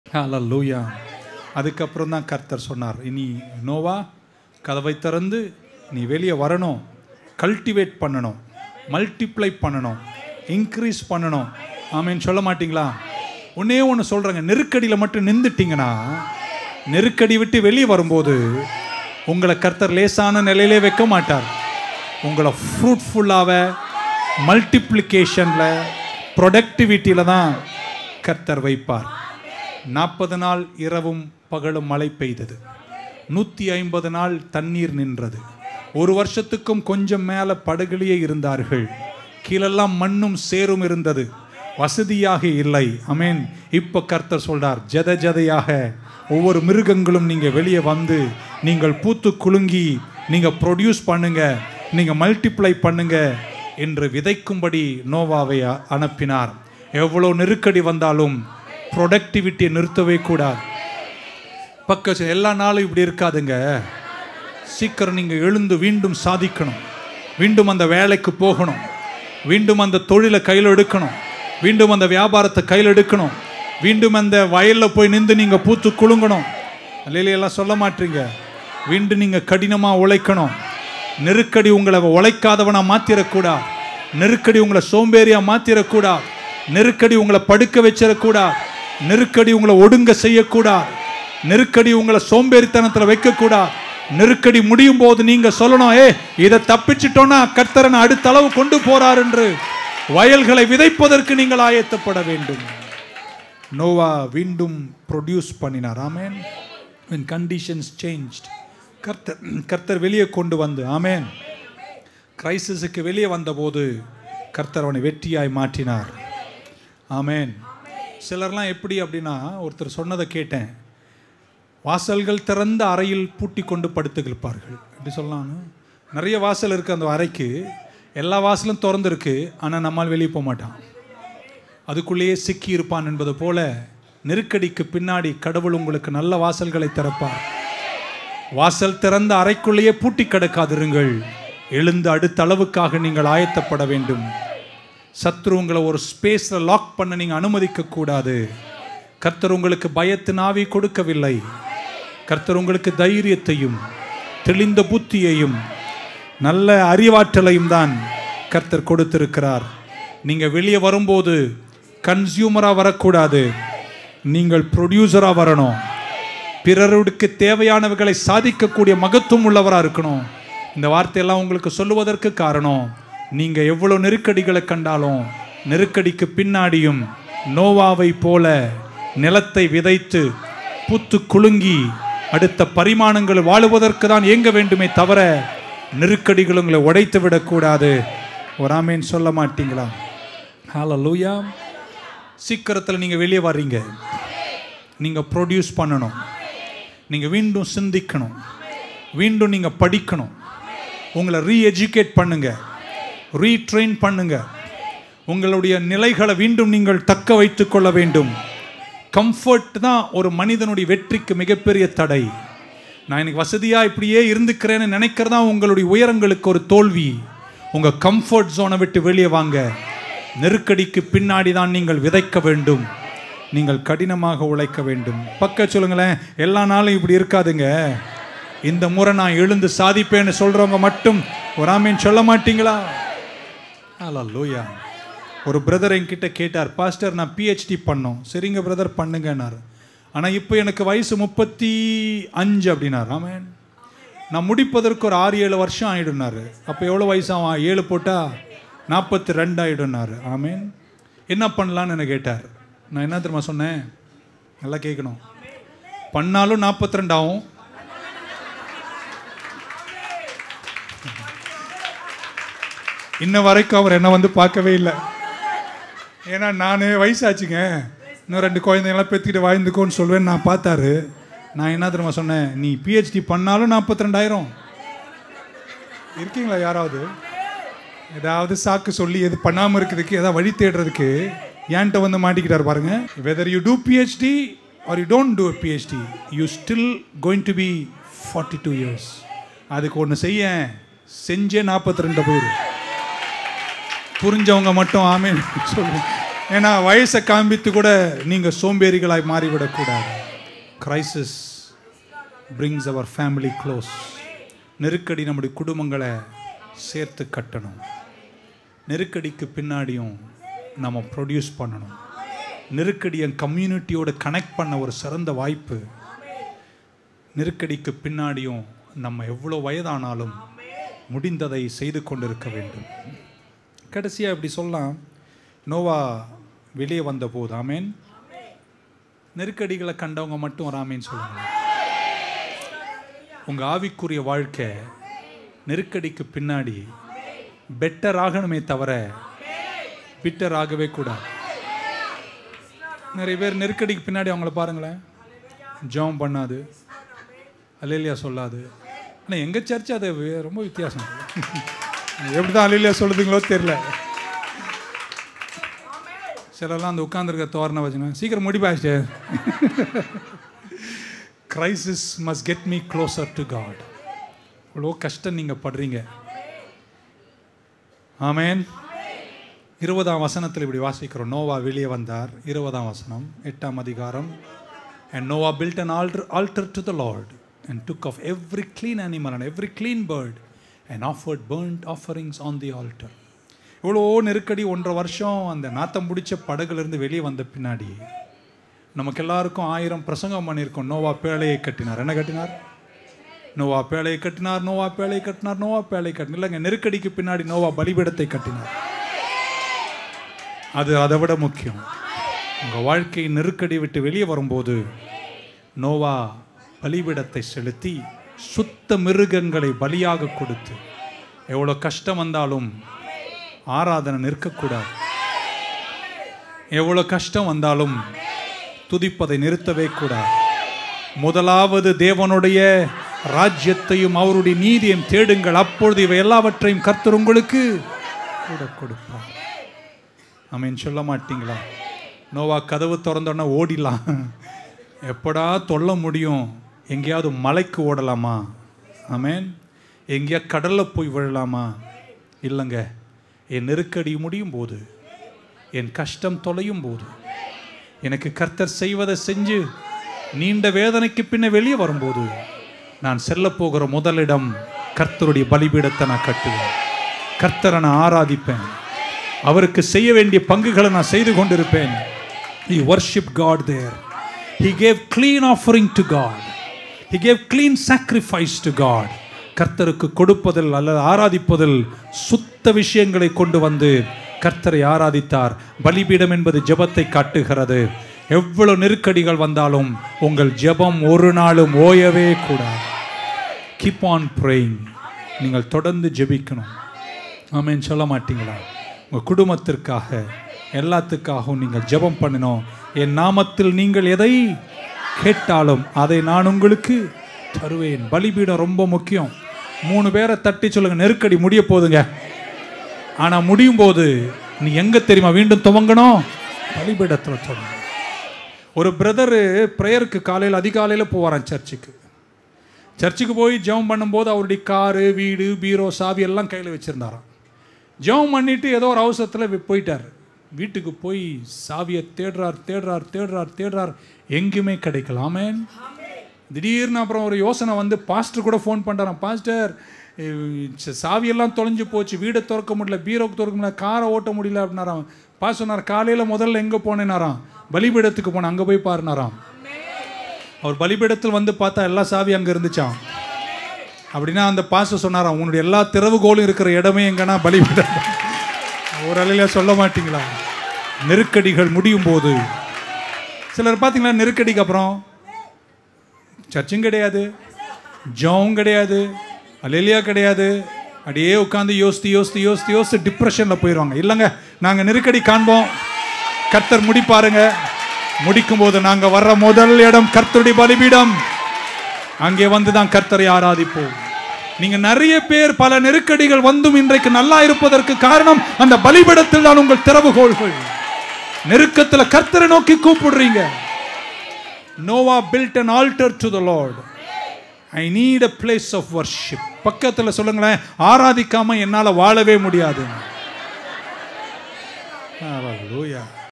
நெருக்கத்திலும் விருத்தியடைய செய்வார் | Pr Jacob Koshy | New Life Ministries|Tamil Christian Message
நீங்கள் எவ்வளவு கடினமான அல்லது மோசமான சூழ்நிலையில் இருக்கலாம் அனாலும் சோர்ந்துபோகாமல் பொறுமையாயிருங்கள்  கர்த்தர் சீக்கிரத்தில் எல்லாவற்றையும் சரி செய்வார்